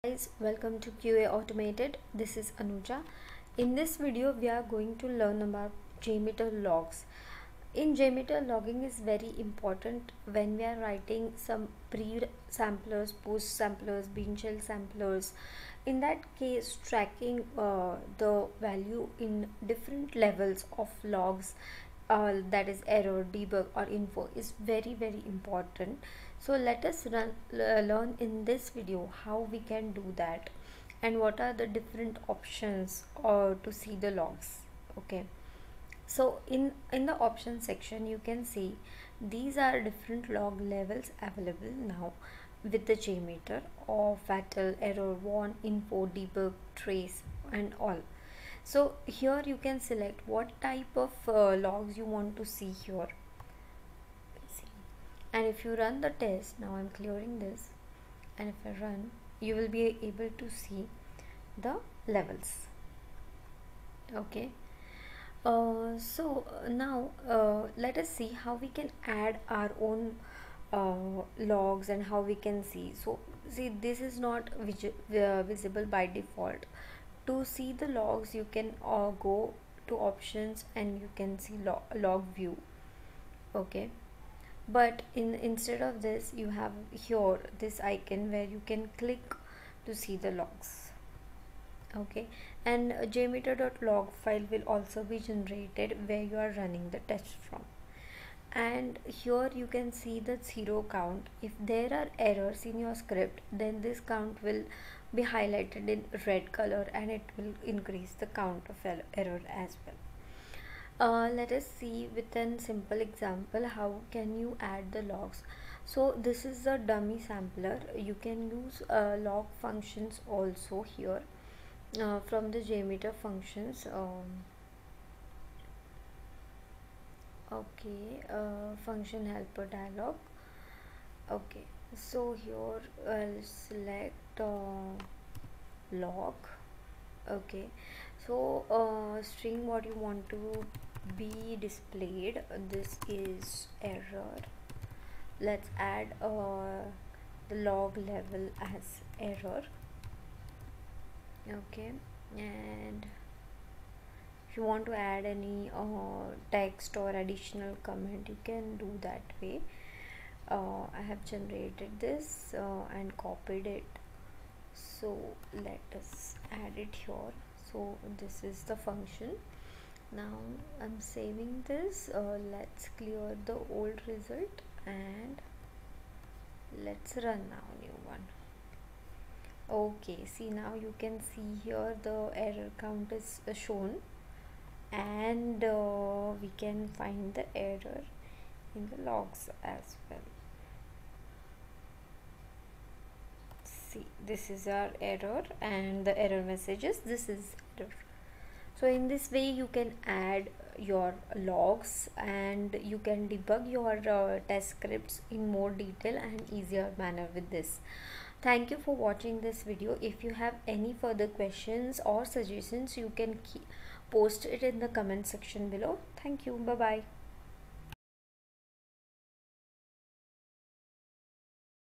Jmeter Tutorial | Jmeter Logging | How to print logs in Jmeter?
In the video we will learn how to add and view logs with Jmeter and Debug the test scripts in more details.

Jmeter Step By Step Tutorial from beginner to advance level. Learn jmeter for performance testing as well as Jmeter For API testing.